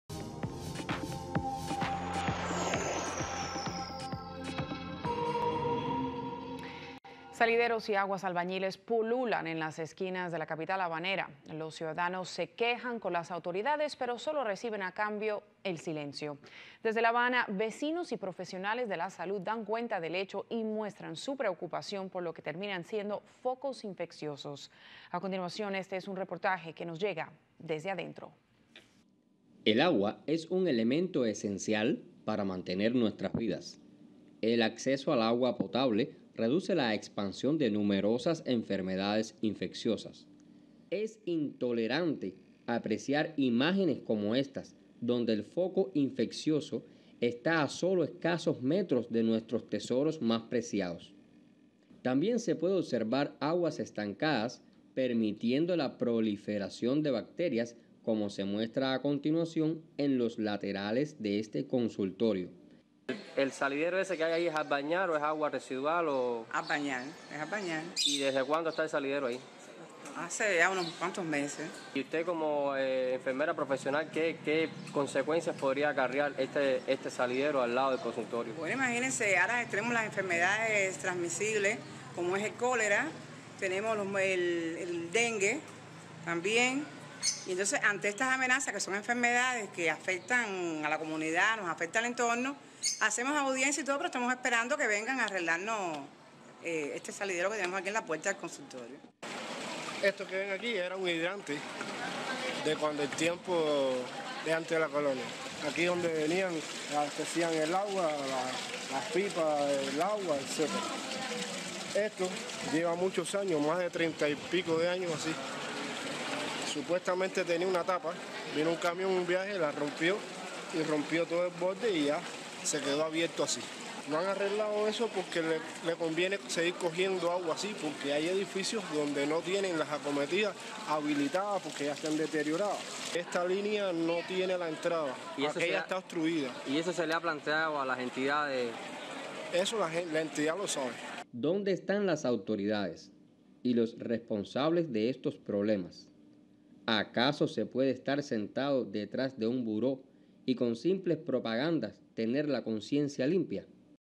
Derrame de aguas albañales y salideros son foco de enfermedades en Cuba
| May. 21 — Salideros y aguas albañales pululan en las esquinas de la capital habanera. Los ciudadanos se quejan con las autoridades pero solo reciben a cambio "el silencio". Desde la habana, vecinos y profesionales de la salud dan cuenta del hecho y muestran su preocupación por lo que terminan siendo "focos infecciosos". A continuación, este es un reportaje que nos llega "Desde Adentro".